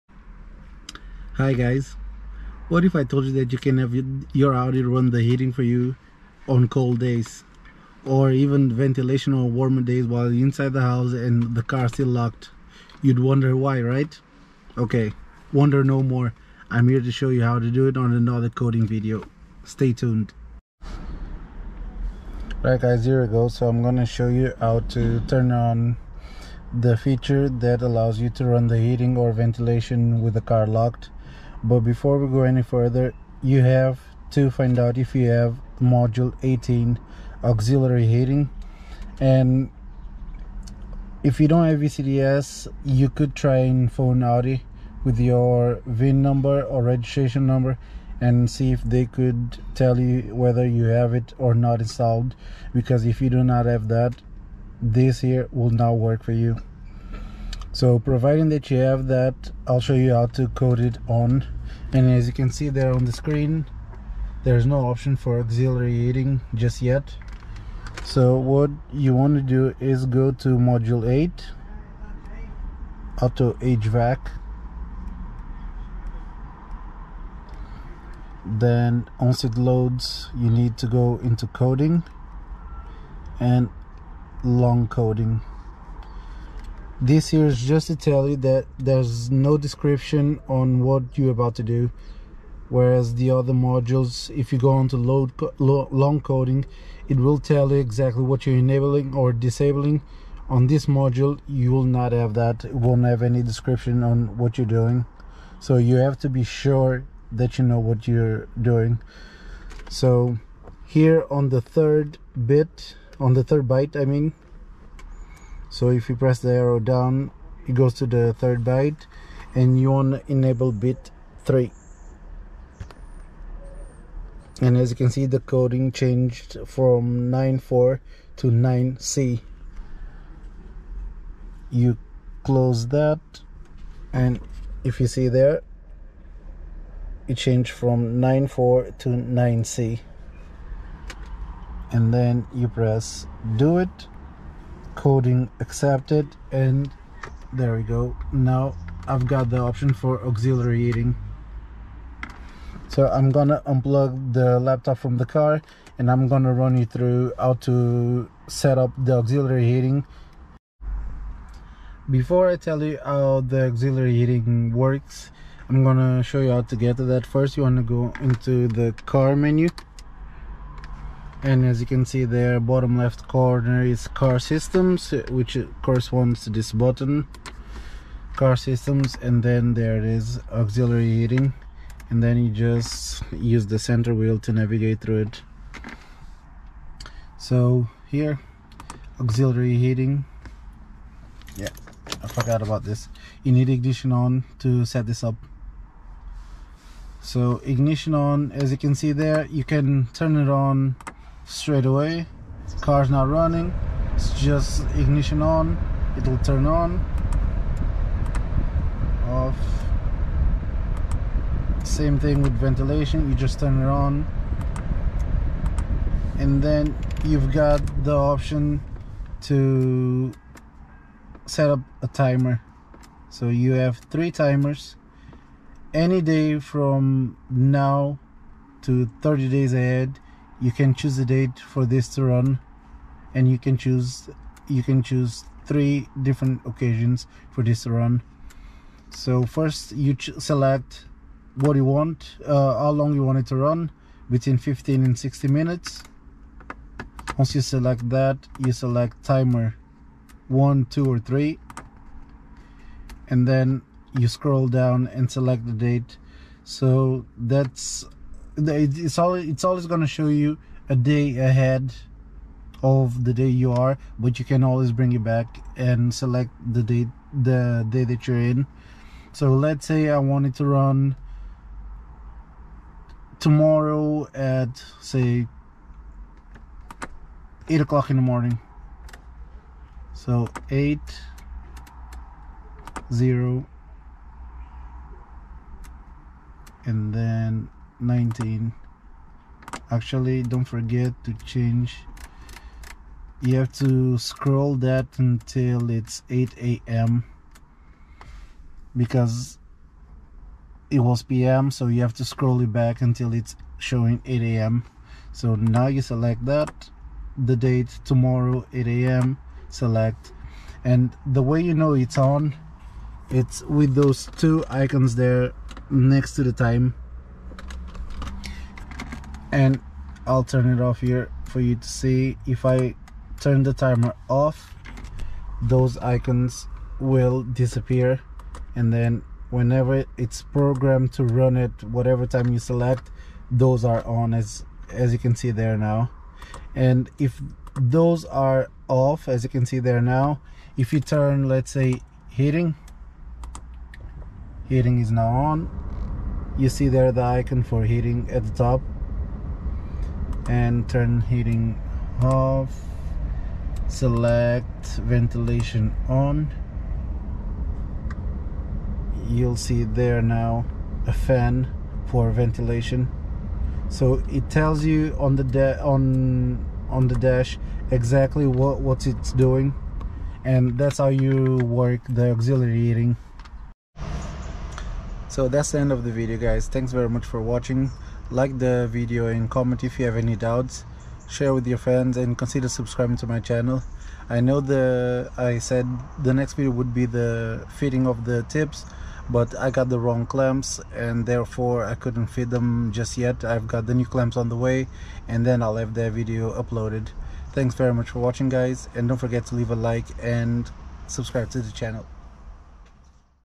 Auxiliary heating enabling through VCDS
In this video I show you how you can have auxiliary heating coded to your car.
This feature even allows you to have a specific remote to turn it on or off from inside the house.
But that requires an additional module and wiring.

PLEASE DO NOT ATTEMPT ANY CONDING UNLESS YOU KNOW EXACTLY WHAT YOU ARE DOING.

0:00 Intro
0:21 Requisits- VCDS
1:09 Coding-VCDS
2:42 Aux. Heat
3:09 How does it work?
6:14 Done and dusted